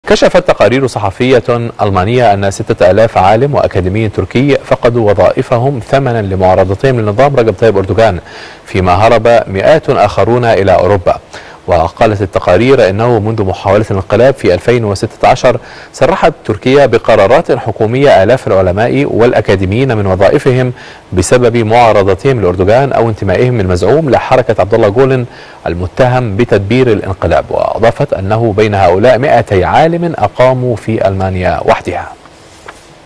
غرفة الأخبار | تقرير: 6000 عالم تركي فقدوا وظائفهم لمعارضة النظام
غرفة الأخبار | تقرير: 6000 عالم تركي فقدوا وظائفهم لمعارضة النظام   | 9 يوليو 2019